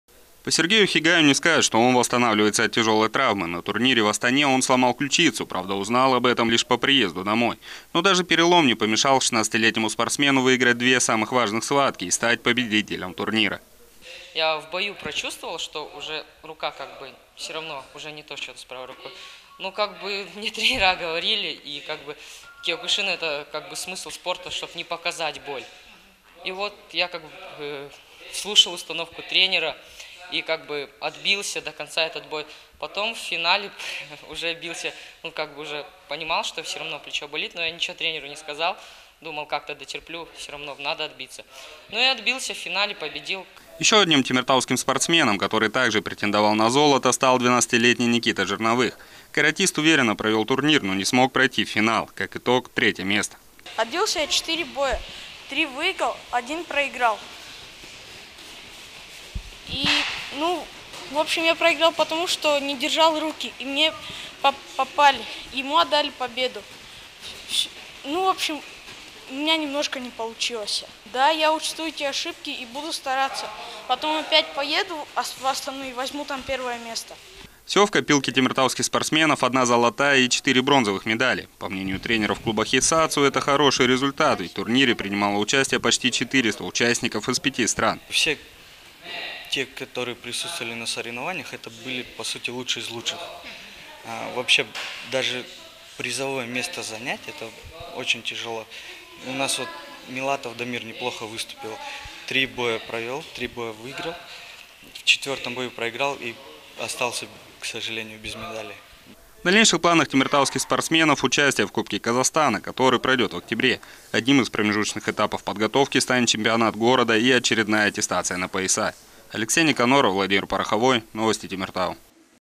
Очередной успех воспитанников «Хисацу»
Темиртауские каратисты успешно выступили на международных соревнованиях, завершившихся на днях в Астане. В копилке юных спортсменов 1 золотая и 4 бронзовых медали.